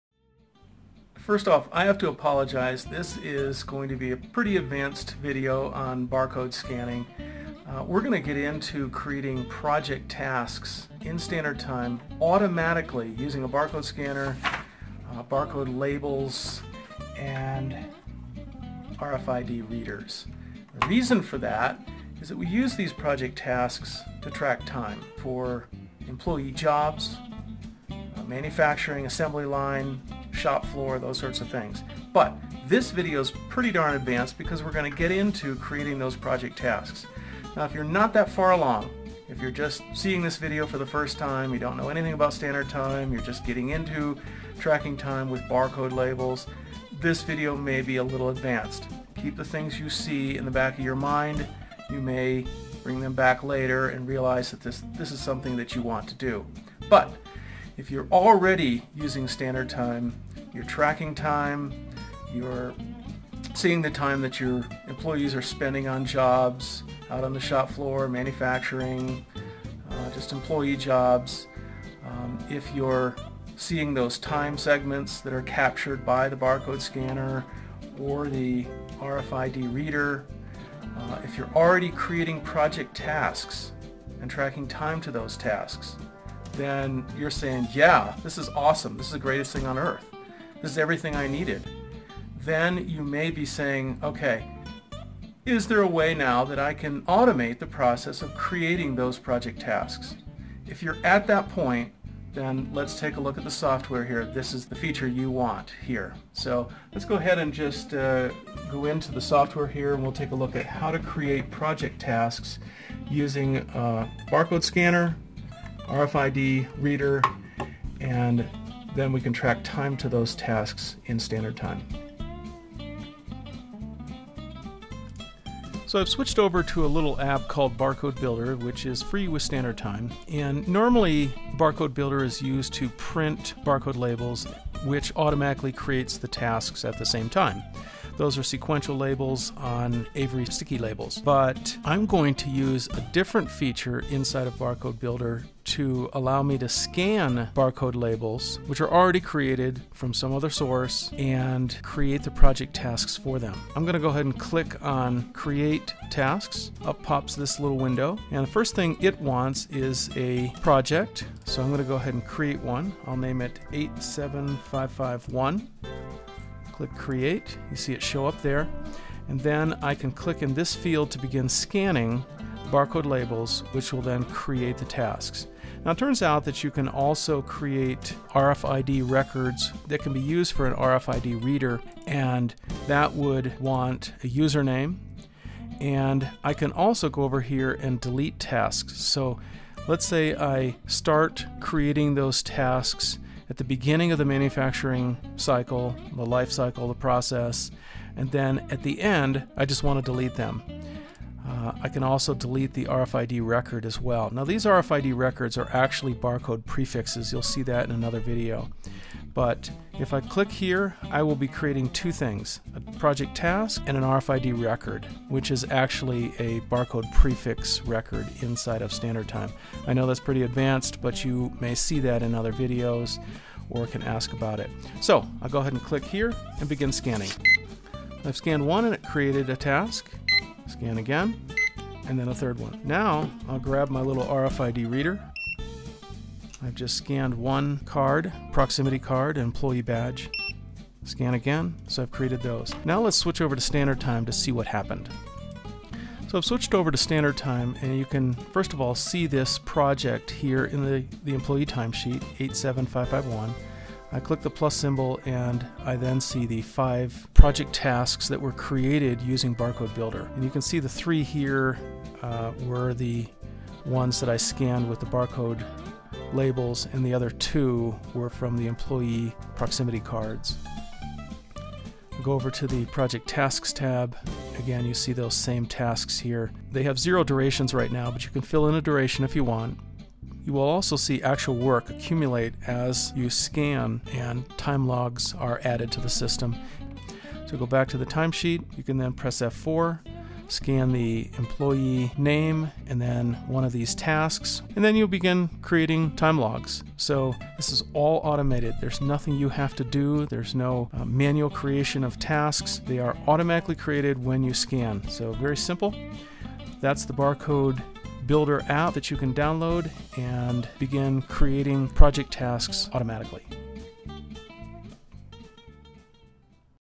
Create Project Tasks from Barcode Labels
First off, I have to apologize, this is going to be a pretty advanced video on barcode scanning. We’re going to get into creating project tasks in Standard Time® automatically; using a barcode scanner, barcode labels and RFID readers. The reason for that is that we use these project tasks to track time for employee jobs, manufacturing, assembly line, shop floor, those sorts of things. This video is pretty darn advanced because we are going to get into creating those project tasks.

If you’re not that far along, if you’re seeing this video for the first time, you don’t know anything about Standard Time, you’re getting into tracking time with barcode labels; this video may be a little advanced. If you’re already using Standard Time, you’re tracking time, you’re seeing the time your employees are spending on jobs, out on the shop floor, manufacturing, employee jobs; if you’re seeing those time segments that are captured by the barcode scanner or the RFID reader. If you’re already creating project tasks and tracking time to those tasks-you’re saying Yeah, this is awesome, this is the greatest thing on earth, this is everything I needed. Then you may be saying OK is there a way now I can automate the process of creating those project tasks. If you’re at that point then let’s take a look at the software, this is the feature you want.

Let’s go into the software here and we’ll take a look on how to create project tasks using barcode scanner, RFID reader and then we can track time to those tasks in Standard Time.

I’ve switched over to an app called Barcode Builder which is free with Standard Time. Normally Barcode Builder is used to print barcode labels which automatically creates the tasks at the same time. Those are sequential labels on Avery sticky labels. But I’m going to use a different feature inside Barcode Builder to allow me to scan barcode labels which are already created from some other source and create the project tasks for them.

First thing it wants is a project, I’ll go ahead and create one. I’ll name it 87551, click create, you’ll see it show up there. Then I can click in this field to begin scanning barcode labels which will then create the tasks. It turns out that you can also create RFID records that can be used for RFID reader and that would want a user name.  And I can also delete tasks; let’s say I start creating those tasks at the beginning of the manufacturing cycle, the life cycle, the process, and then at the end I want to delete them. I can also delete the RFID record as well. Now these RFID records are actually barcode prefixes, you’ll see that in another video. But if I click here I’ll be creating two things; project task and a RFID record. Which is actually a barcode prefix record inside of Standard Time. I know that’s pretty advanced but you may see that in other videos or can ask about it.

I’ll click here and begin scanning. I’ve scanned one and created a tasks, scan again and a third one. I’ll grab my RFID reader, I’ve just scanned one card, proximity card, employee badge, scan again I’ve created those.

Let’s switch over to Standard Time to see what happened. You can first of all see this project in the employee timesheet, 87551. I click the + symbol and I then see the five project tasks that were created using barcode builder. You can see the three here were the ones I scanned with the barcode labels and the other two were from the employee proximity cards. Go over to the project tasks tabs again you’ll see the same tasks here. They have zero duration’s right now but you can fill in the duration if you want. You will also see actual work accumulate as you scan and time logs are added to the system.

Go back to the timesheet you can press F4, scan the employee name and then one of these tasks. Then you’ll begin creating time logs.

This is all automated, there’s nothing you have to do, there’s no manual creation of tasks. They are automatically created when you scan. Very simple. That’s the Barcode Builder app that you can download and begin creating project tasks automatically.